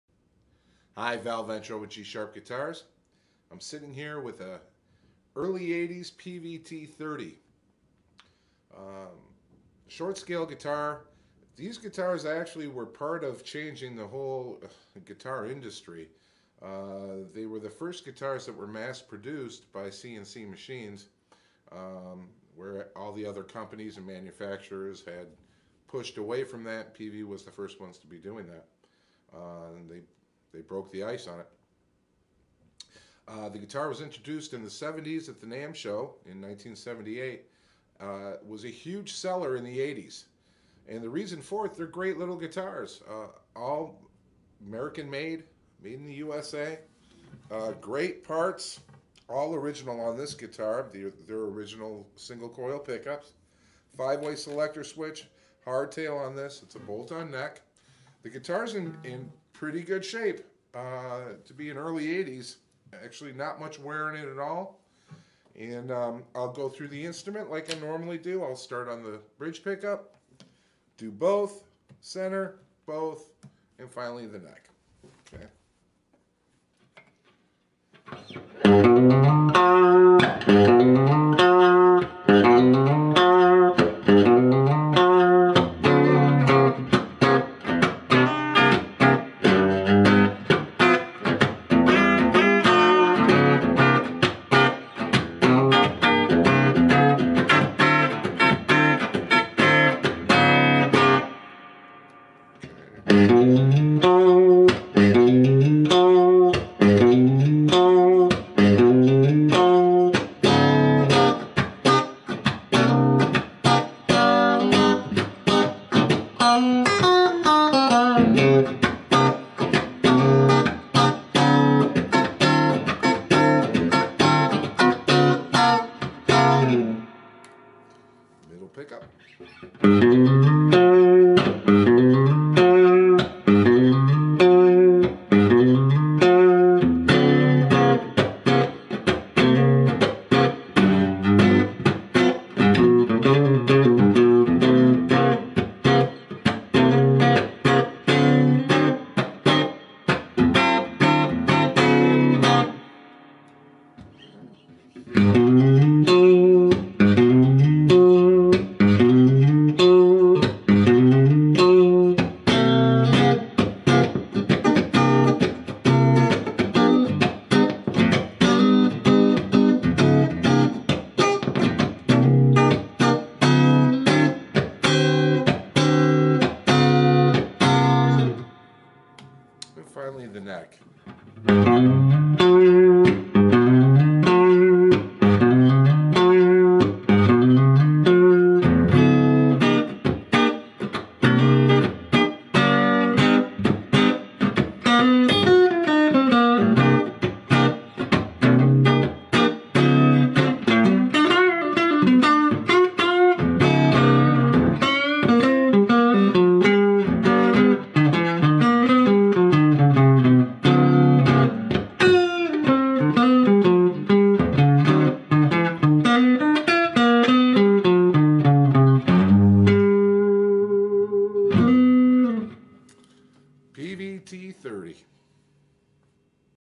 G-Sharp Guitars - 1980's Peavey T30 031
G-Sharp Guitar-1980's Peavey T30,
Val Ventro shows you an 80's Peavey T30 guitar, now for sale at G-Sharp Guitars.
3/4 scale, three single-coil pickups, bolt-on maple neck, five-way switch.
Hrs: Saturday, 10am-4pm   Wednesday, 4pm-8pm
Recorded on a Sony HDR-MV1